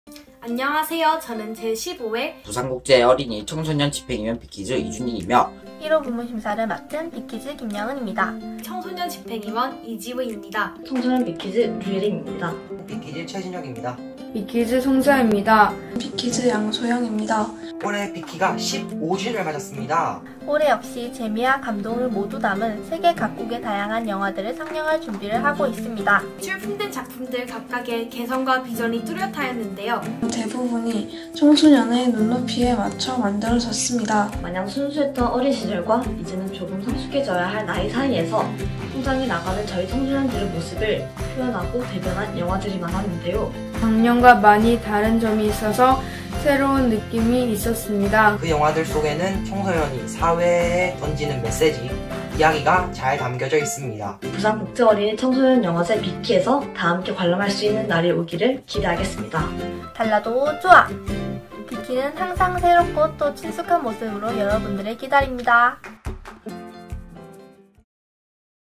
[2020 15th BIKY] "레디~액션! 15" 예선심사평 / 'Ready~ Action! 15' Commentary from BIKies!
비키의 경쟁부문 '레디~액션!'은 어린이 청소년 감독들이 직접 제작한 영화들로 이루어져 있습니다! 경쟁부문의 예선심사도 역시 어린이 청소년들이 진행한다는 사실 알고 계셨나요? 어린이 청소년 집행위원 비키즈(BIKies)의 심사평을 공개합니다~!

BIKY's competition section 'Ready~ Action!' consists of films made by kids and youth filmmakers! Did you know that the jury of preliminary round is also kid and youth? BIKies, the kids and youth executive committee of BIKY gave a short review of this years' 'Ready~ Action!' section!